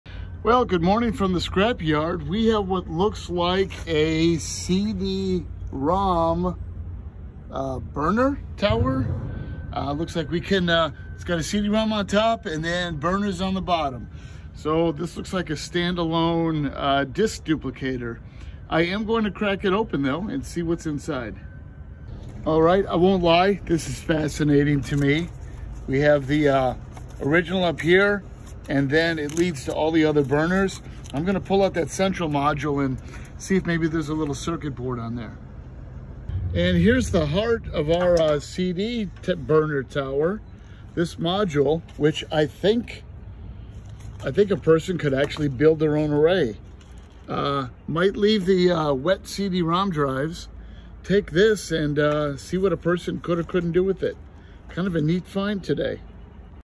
VINPOWER SharkCopier Standalone Optical CD-ROM Burner Tower
Rescued the control module from an old CD-ROM copier system. Pretty neat!